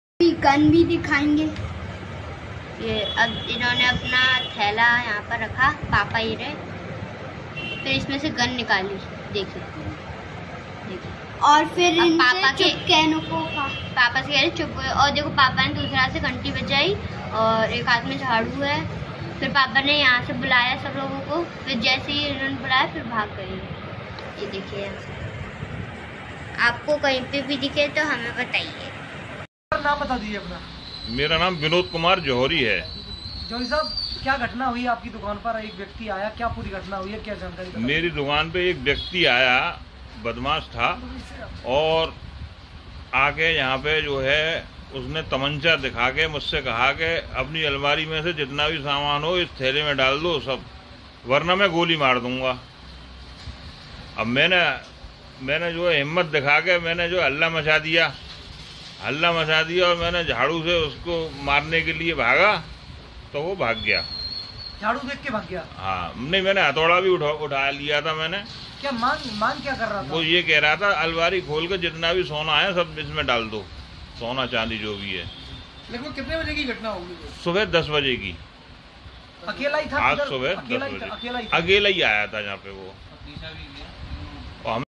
आगरा : लूट करने ज्वेलरी शॉप में घुसे बदमाश ने दिखाया तमंचा, कारोबारी ने झाड़ू से पीटकर भगाया
आगरा में लूट करने ज्वेलरी शॉप में घुसे बदमाश ने दिखाया तमंचा, कारोबारी ने झाड़ू से पीटकर भगाया; CCTV फुटेज आया सामने